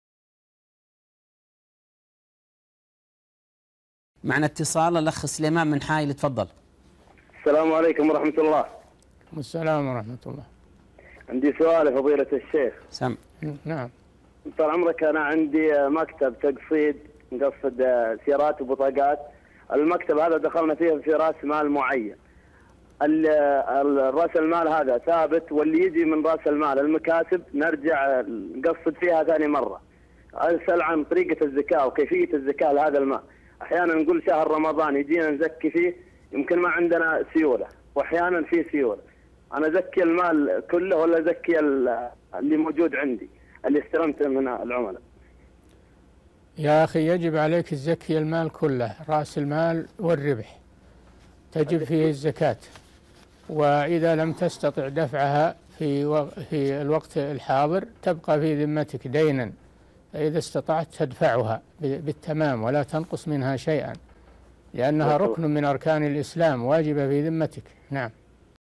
هل يخرج الزكاة عن الأرباح فقط أم عن رأس المال أيضاً؟
-نص السؤال: المتصل سليمان من حائل يقول: أنا عندي مكتب تقسيط ودخلنا هذا المكتب برأس مال معيّن، رأس المال هذا ثابت والذي يأتي من رأس المال المكاسب نرجع نقسط فيها ثاني مرة، أسأل عن طريقة الزكاة لهذا المال، أحياناً نقول شهر رمضان وعندما يأتي أحياناً لا يكون عندنا سيولة، فهل أزكي عن المال كامل أم عن الموجود عندي الذي استلمته من العملاء؟
السيرة الذاتية :
للاشتراك في قناة الشيخ صالح الفوزان
لمشاهدة حلقات فتاوى على الهواء كاملة

رئيس الفريق
لمشروع كبار العلماء